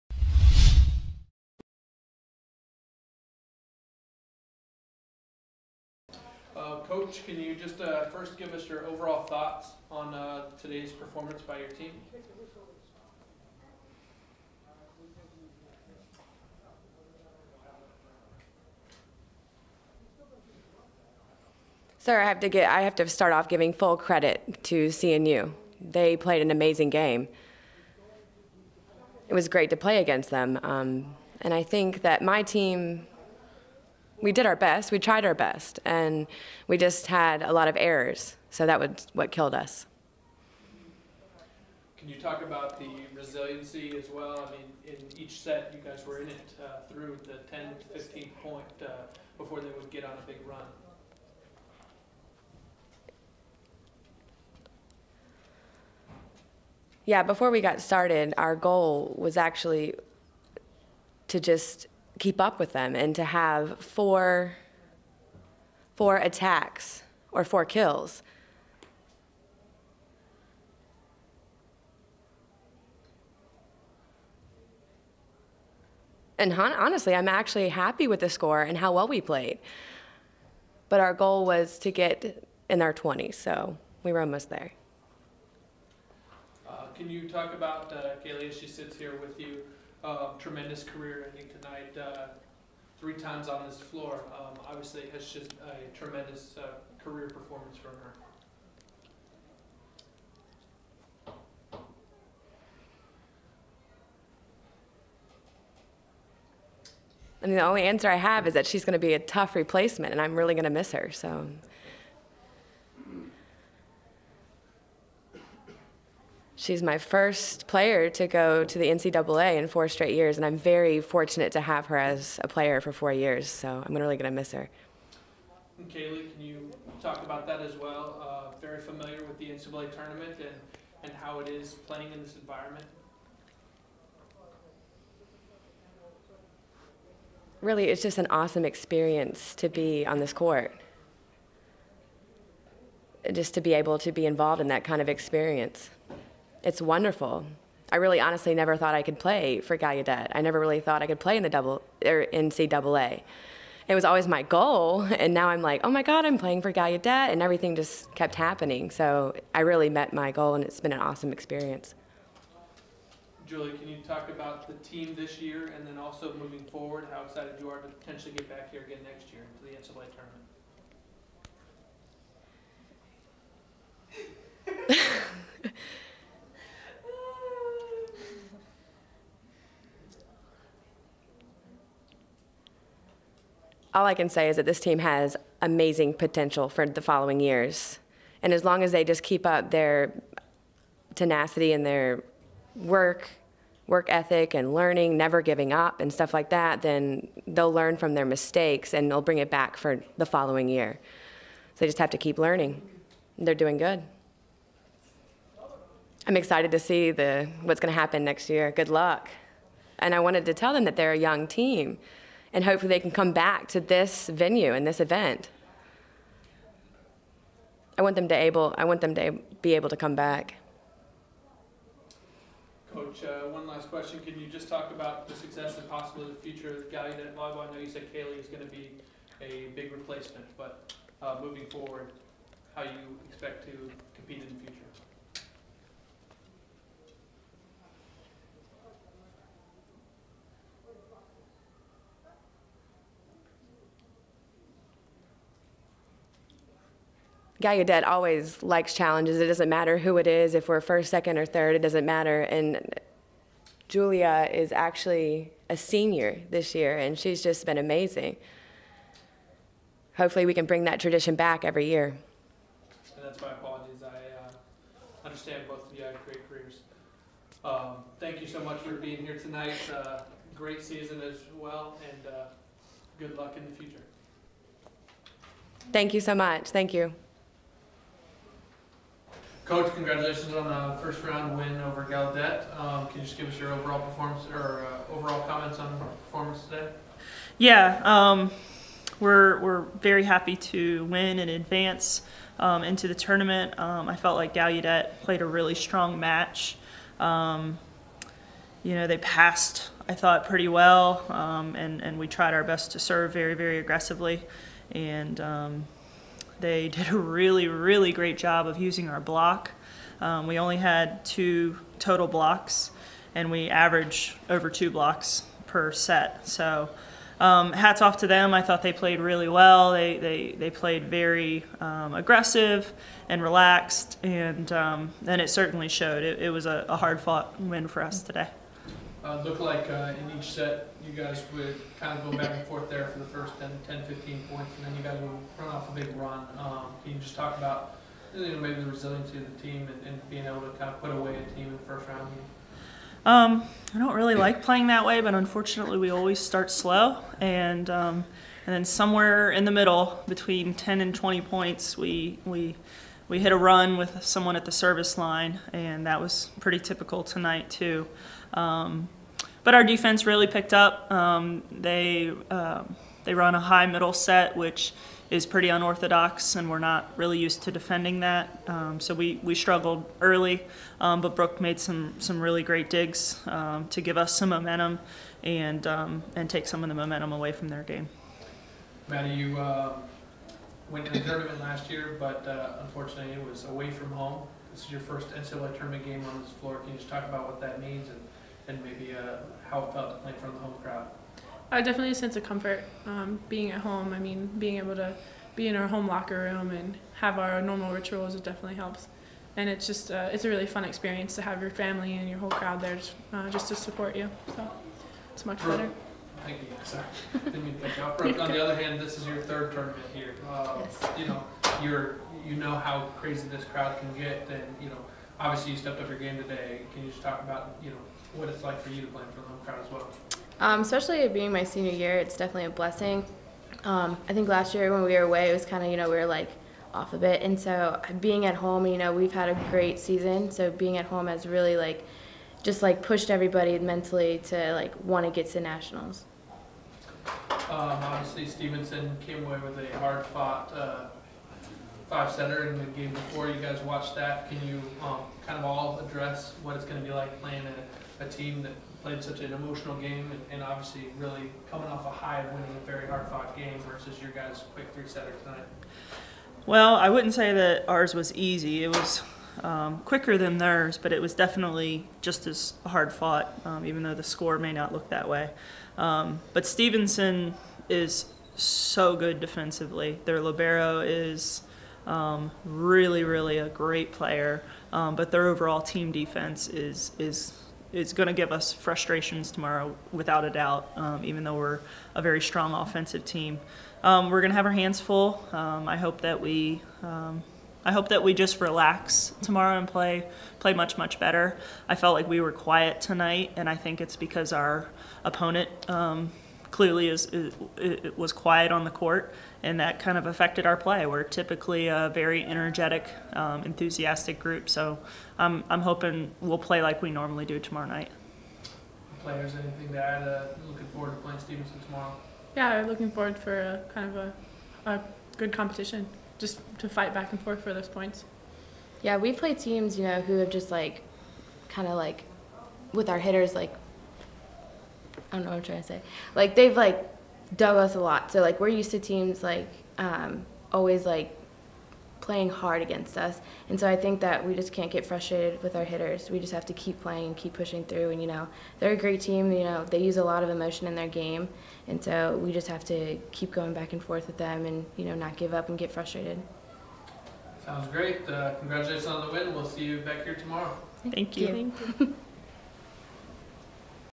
NCAA Volleyball Regionals Round 1 Gallaudet vs Christopher Newport Univ Post Game Press Conference
Christopher Newport Univ Defeats Gallaudet 3-0 in NCAA Volleyball Regionals to advance to the next round.